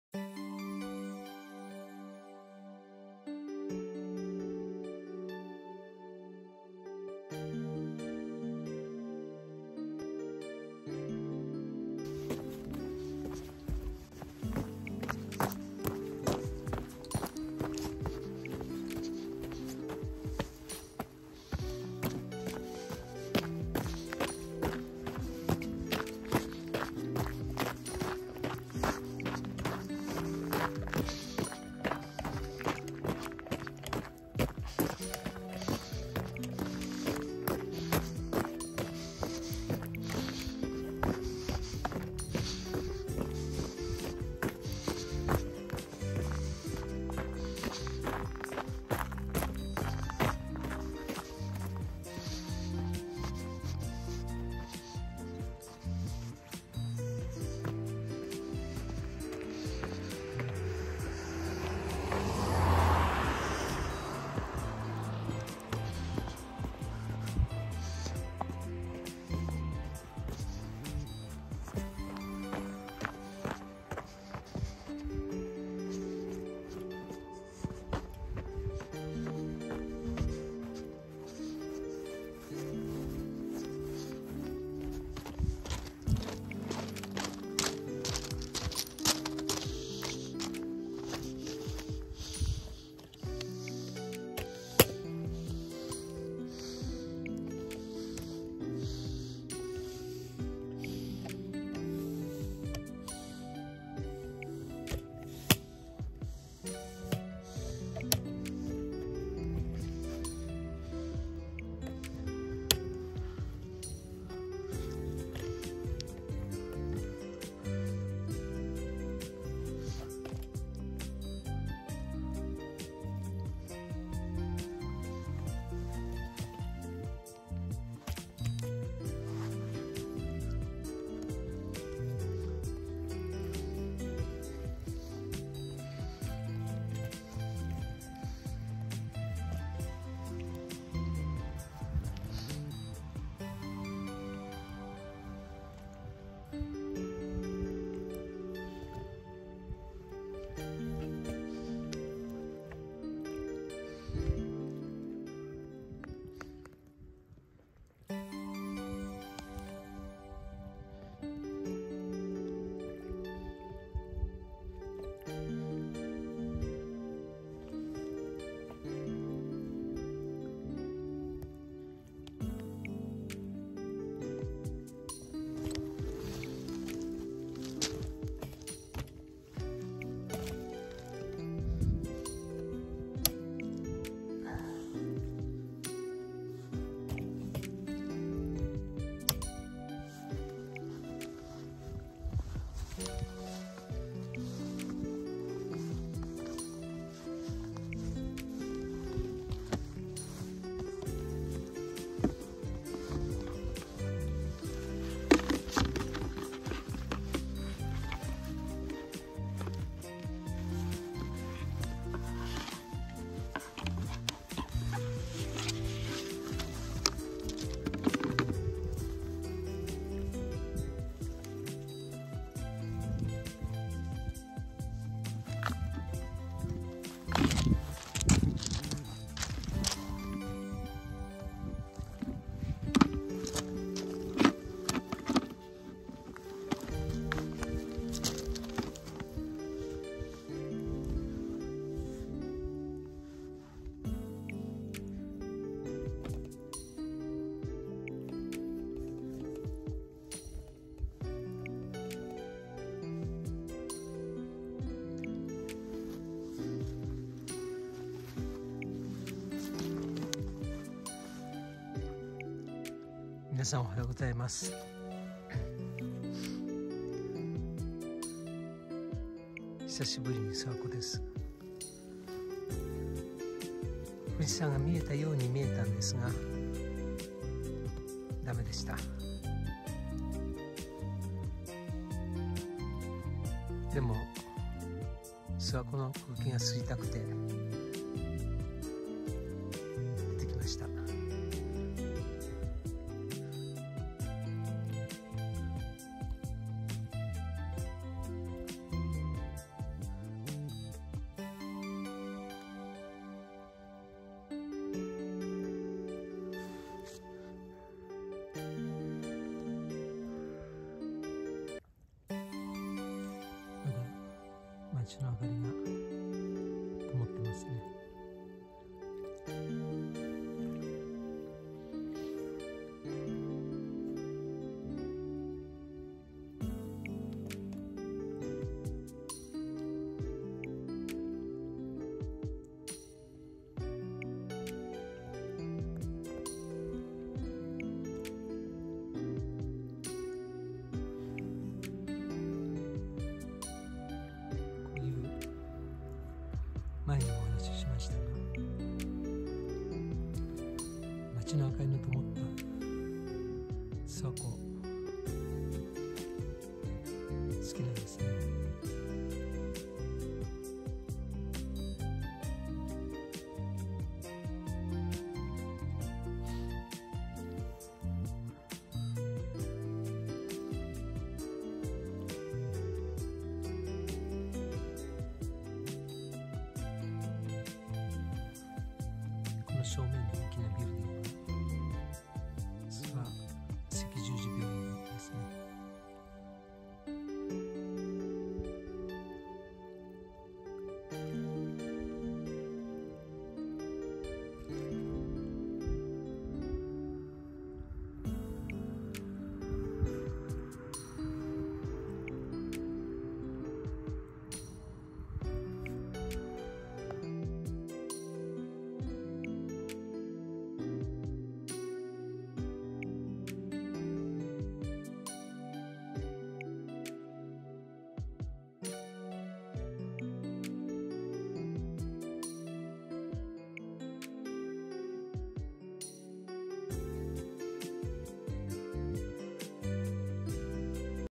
2022/3/21 夜明け前の美しい諏訪湖と富士山（見えません）Lake Suwa and Mt. Fuji before dawn (not visible)【Relaxation4K】
夜明け前の美しい諏訪湖と富士山（見えません）
Beautiful Lake Suwa and Mt. Fuji before dawn (not visible)
撮影日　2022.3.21  Shooting date 2022,March21
撮影場所　長野県諏訪郡下諏訪町　「関東の富士見百景」に選ばれた地点より450m西「一つ浜公園」前 Location: Shimosuwa-cho, Suwa-gun, Nagano Prefecture, 450m from the point selected as "100 Views of Fujimi in Kanto" in front of「 Hitotsuhama Park」 国土交通省　関東地方整備局の「関東の富士見百景」 "100 Views of Fujimi in Kanto" by the Kanto Regional Development Bureau, Ministry of Land, Infrastructure, Transport and Tourism 使用BGMはVLLO著作権フリー音源であり、著作権上何ら問題ありません。 The BGM used is a VLLO copyright-free sound source, and there is no problem with copyright.